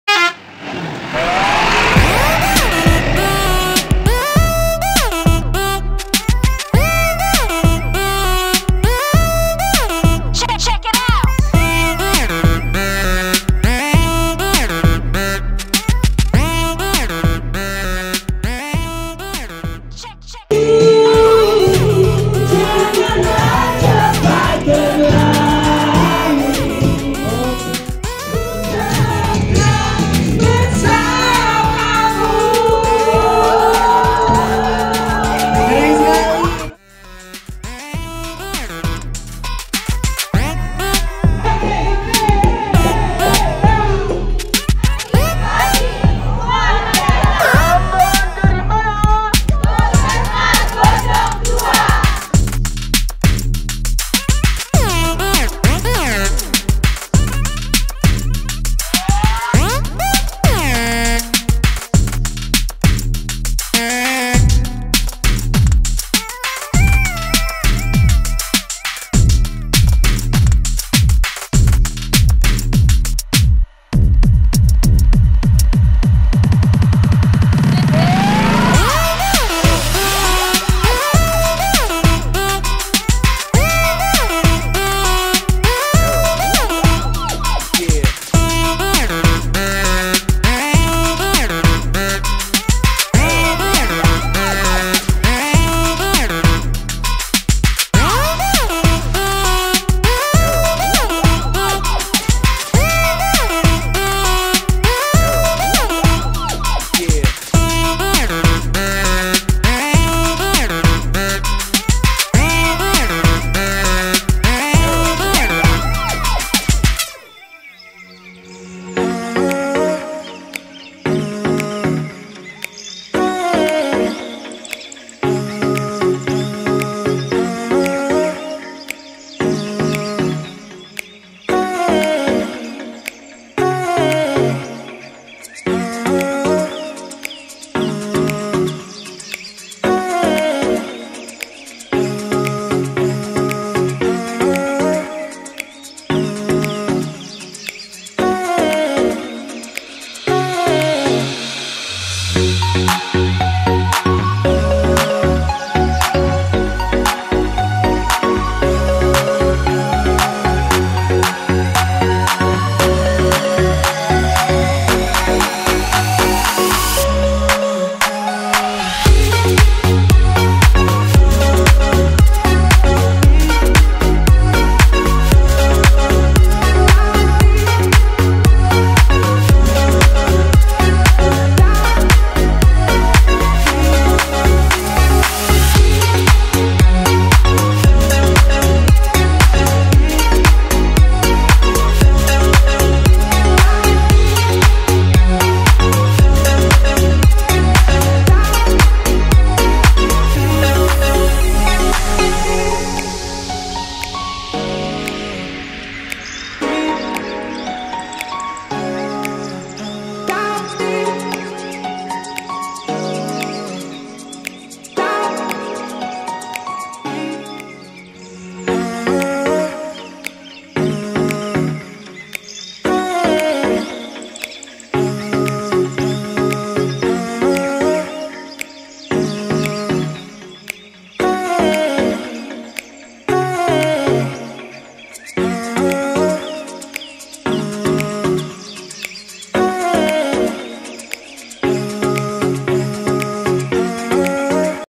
wisata Bandung||Tangkuban Perahu_Outbound Grafika Cikole_Great Asia Afrika
highlight video kegiatan employee gathering puskesmas Bojong II kec. Bojong kabupaten pekalongan di kawasan lembang Bandung...

kamantara tour & travel

kamantara_official

kamantara tour official